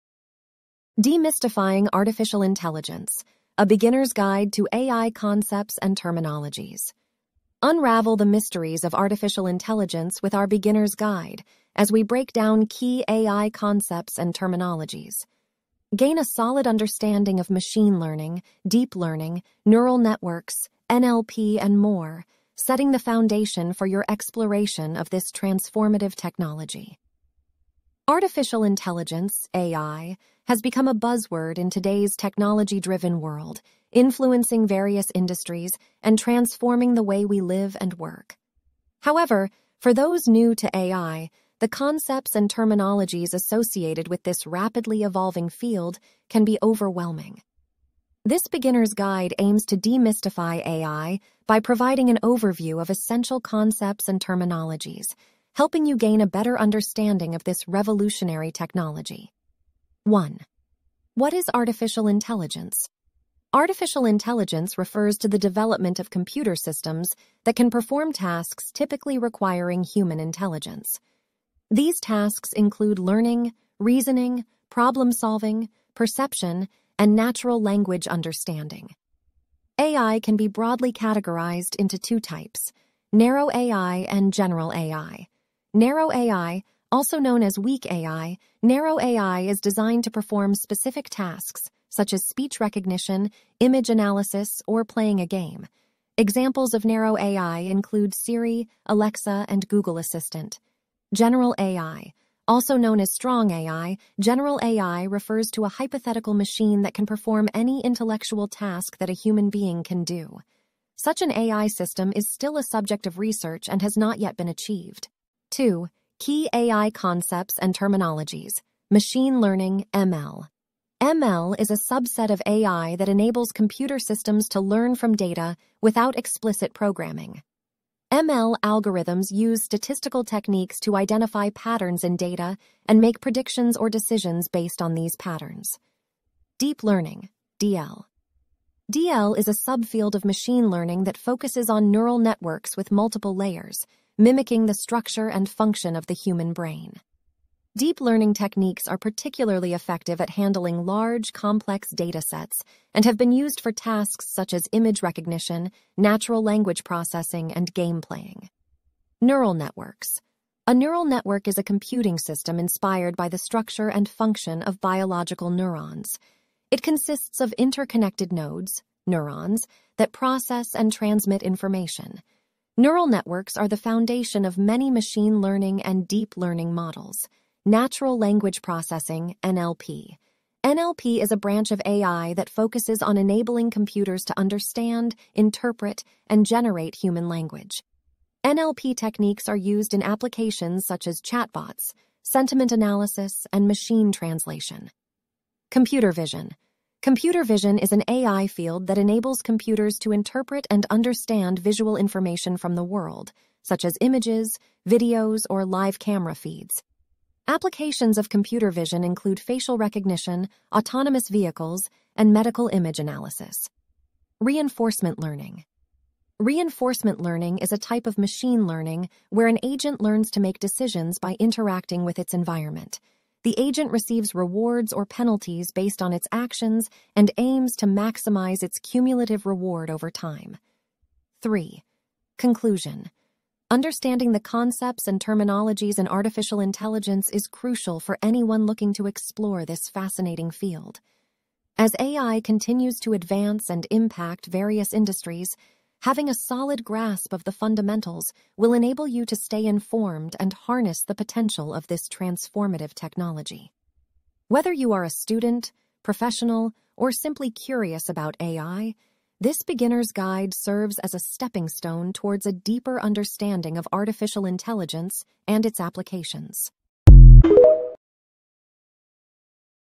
Demystifying Artificial Intelligence: A Beginner's Guide to AI Concepts and Terminologies
Unravel the mysteries of Artificial Intelligence with our beginner's guide, as we break down key AI concepts and terminologies. Gain a solid understanding of machine learning, deep learning, neural networks, NLP, and more, setting the foundation for your exploration of this transformative technology.